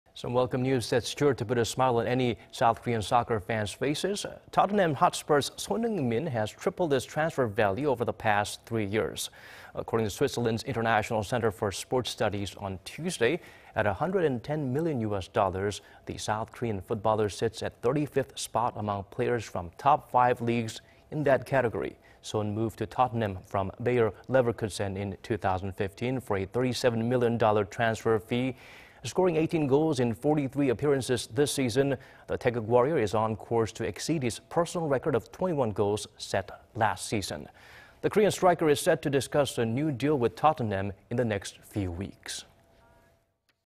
Tottenham Hotspur's Son Heung-min triples his transfer value in 3 years
손흥민 예상 몸값 1천억 돌파…3년 만에 세 배

  Tottenham Hotspur's  Son Heung-min... has tripled his transfer value over the past three years.
  According to Switzerland's International Center for Sports Studies on Tuesday,… at a hundred-and-10 million US dollars, the South Korean footballer sits at 35th spot among players from top five leagues in that category.
Son moved to Tottenham from Bayer Leverkusen in 2015 for a 37 million dollar transfer fee.
Scoring 18 goals in 43 appearances this season, he is on course to exceed his personal record of 21 goals, set last season.
The Korean striker is set to discuss a new deal with Tottenham in the next few weeks.